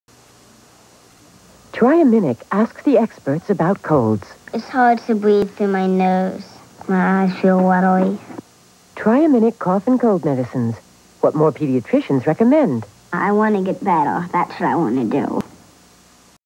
Triaminic Cold Syrup Commercial (1988)
A little girl tells us why Triaminic is her choice for cold syrup.